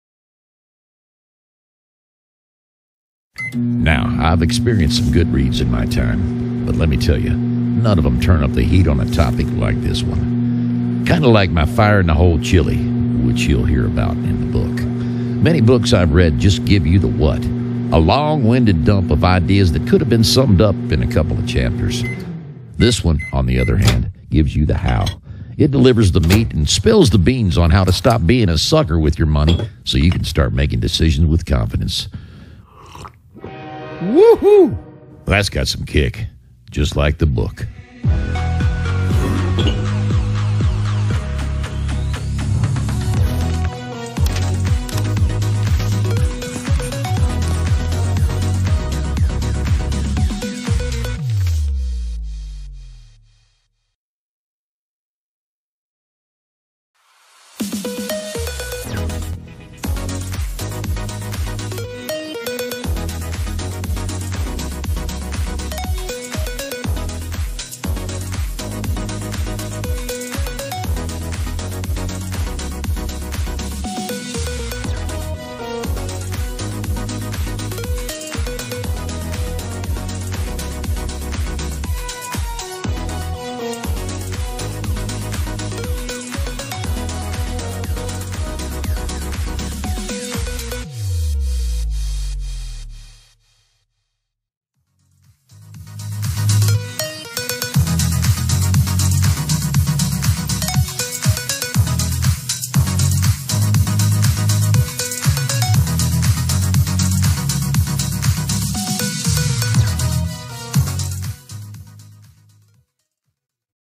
George heats things up. See what's cooking!
George knows when a book is useful and when it's just full of it. Hear him out, over a bowl of his famous chili.

(TLDR - watch all 5 videos)